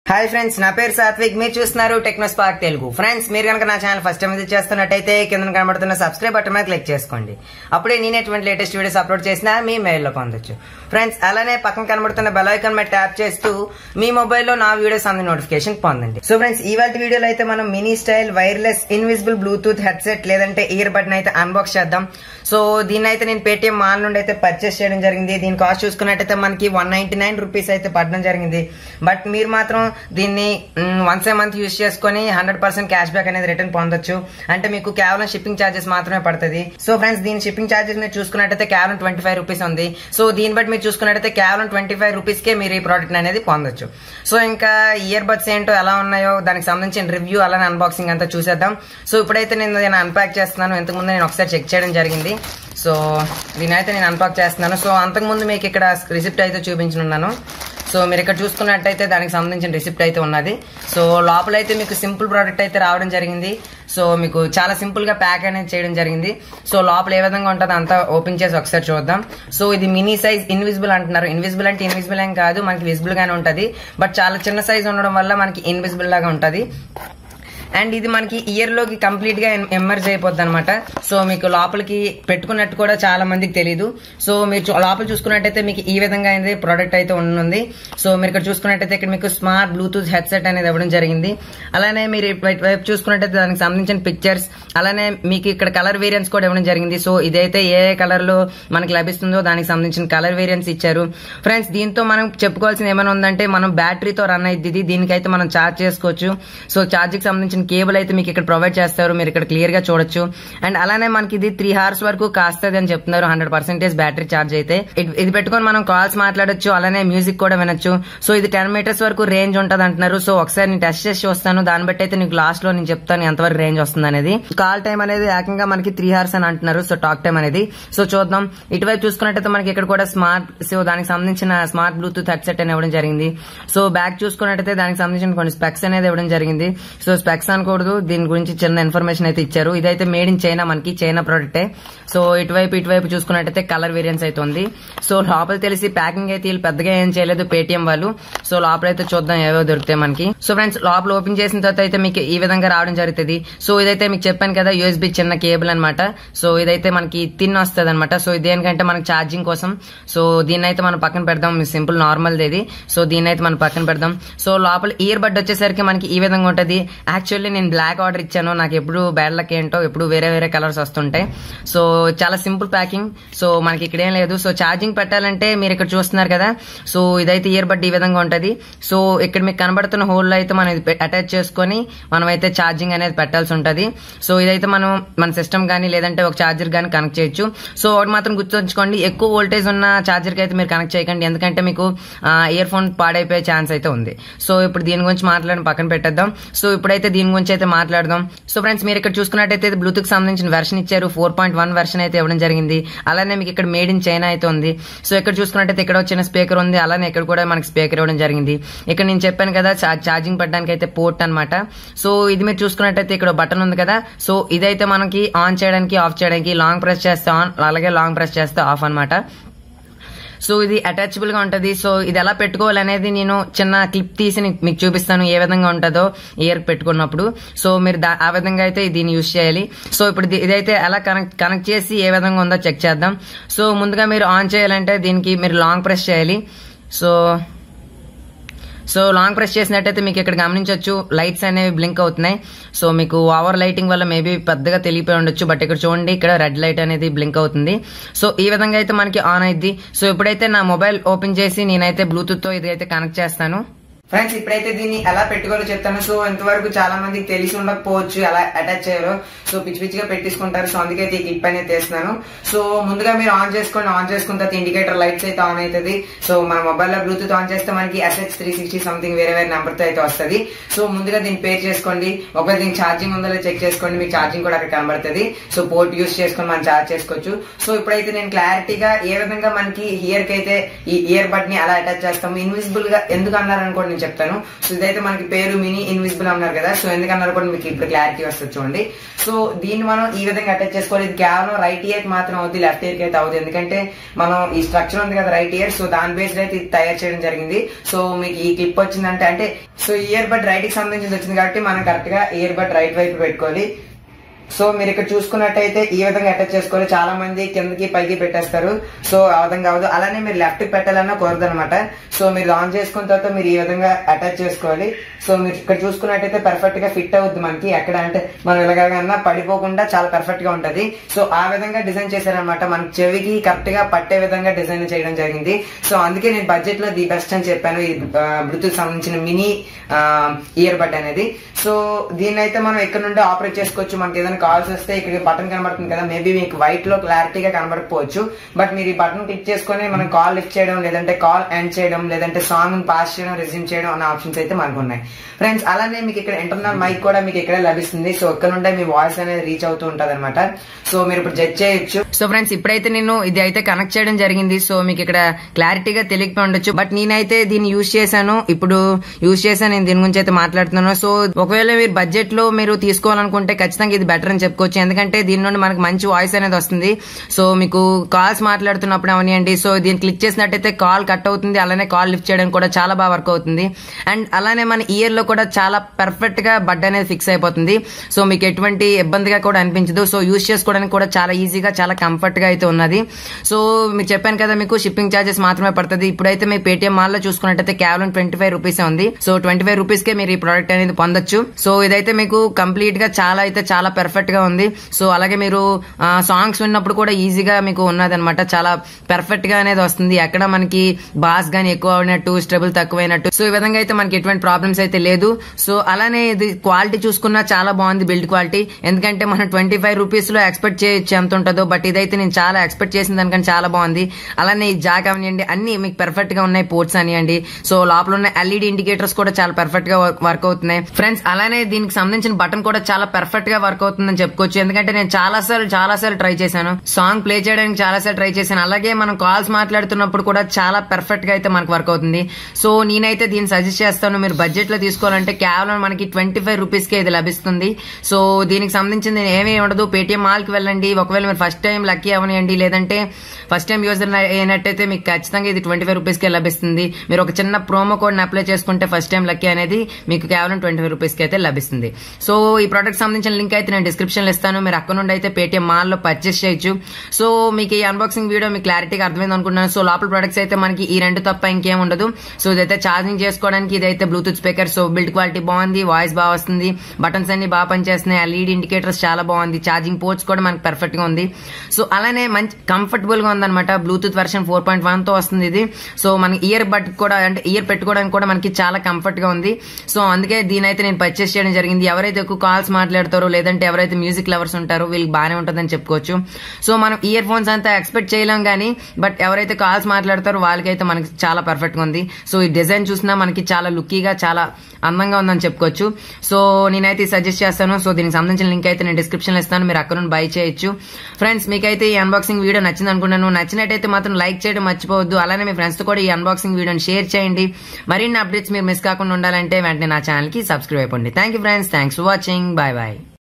#5 Unboxing and Review Mini Invisible Bluetooth Headset(earbud)
Hope u understood.

If u like this video please like...

If u feel this is Useful plz share...

Product link:Check out this product on Paytm!